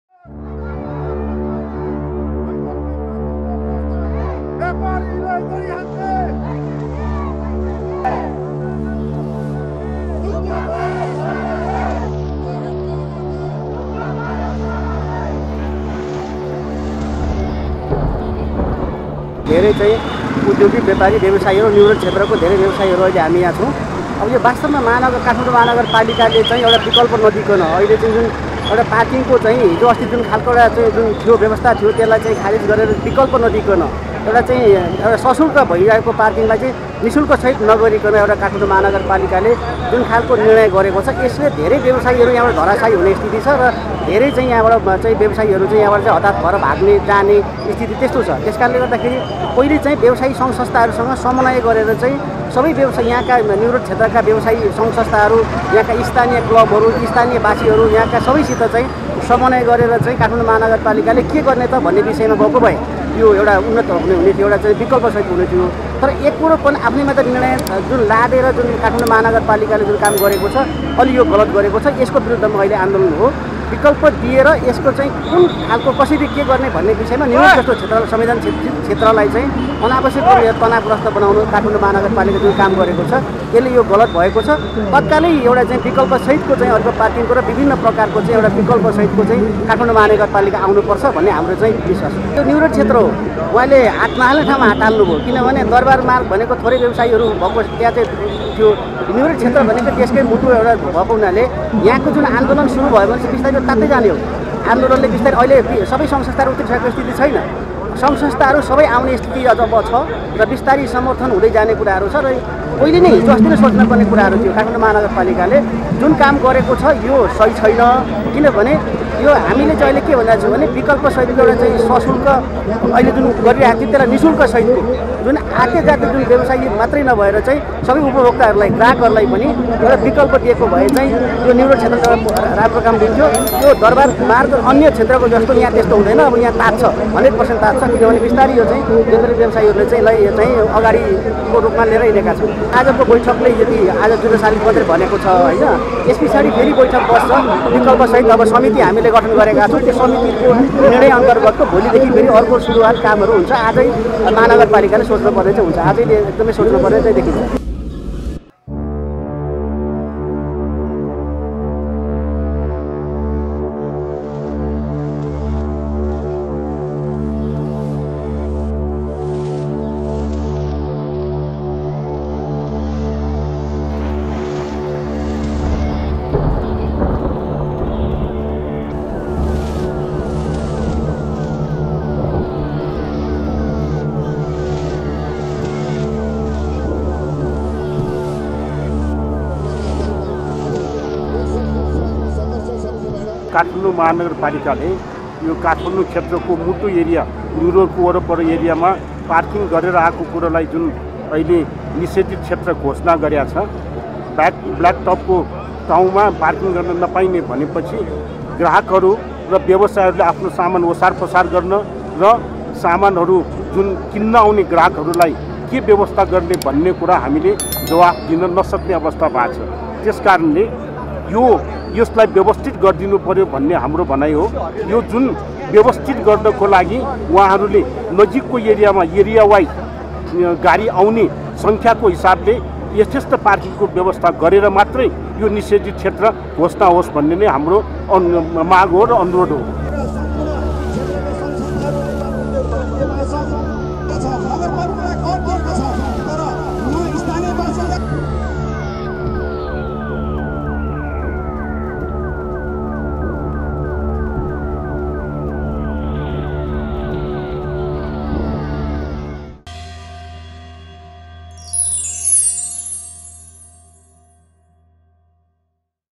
महानगरबिरुद्ध न्युरोड क्षेत्रका व्यवसायी दोस्रो दिन पनि आन्दोलित, निर्णय फिर्ता लिन माग | Newroad
काठमाडौं महानगरपालिकाले गरेको न्युरोड क्षेत्रमा पार्किङ निषेधको बिरोधमा व्यवसायीहरू दोस्रो दिन पनि आन्दोलित भएका छन् ।
महानगरपालिकाले मंगलवारदेखि लागू हुनेगरी धर्मपथ, जुद्ध शालिकदेखि इन्द्रचोक तर्फको सडकखण्ड, खिचापोखरी, महाबौद्ध, वीर अस्पताल वरपरको कालोपत्र सडकमा पार्किङ निषेध गरेको थियो । उक्त क्षेत्रका व्यवसायीहरुले महानगरले व्यवसायीहरुसँग समन्वय र पार्किङको विकल्प बिना एकतर्फी रुपमा निर्णय गरेको भन्दै तत्काल निर्णय फिर्ता लिन माग गरेका हुन् ।